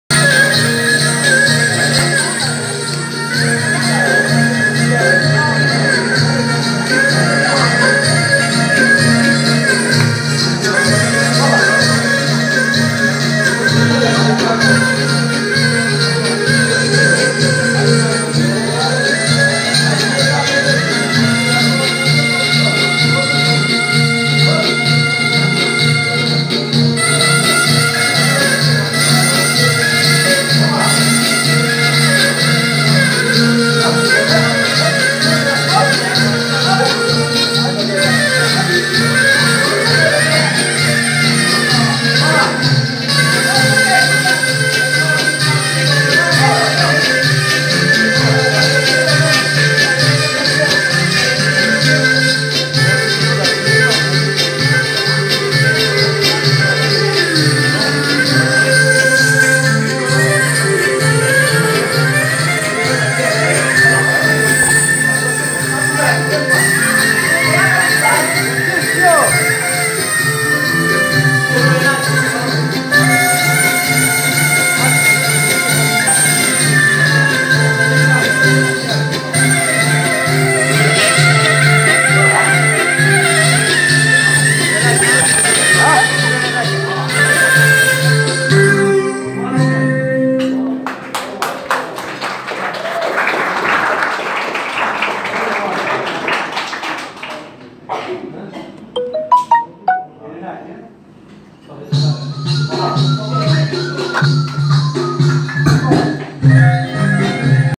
Zeri i Bilbilit
Nata Shqiptare me turista Suedes.Ansambli "Zeri i Bilbilit"filloj sezonin turistik me nje shfaqje te bukur me  kenge polifonike dhe valle tradicionale.Ju urojm nje sezon te sukseshem.